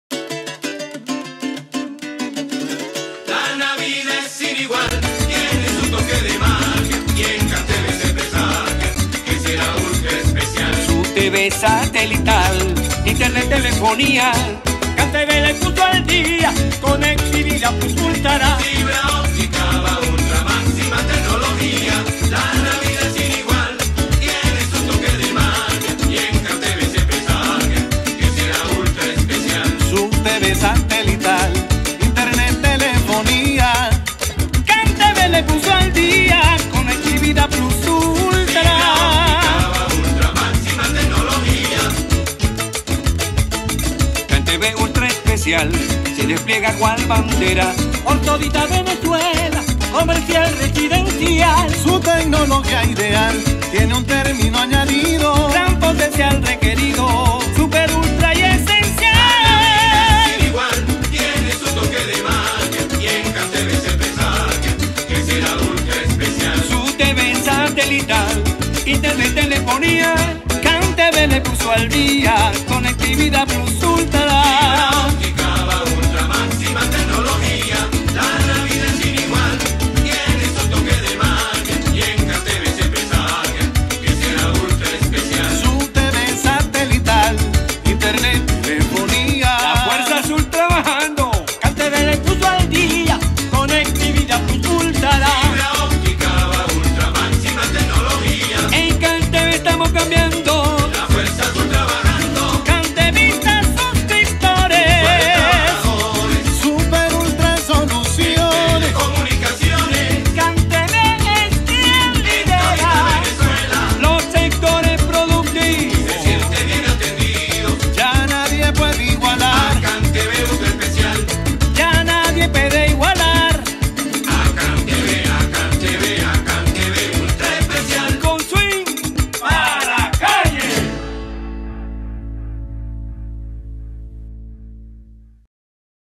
Navidad Ultra Especial de Cantv se despliega por todo el país
En Cantv celebramos la mejor época del año, deseándole a todas las familias una Navidad Ultra Especial. Promovamos juntos los valores y tradiciones que nos identifican como venezolanos.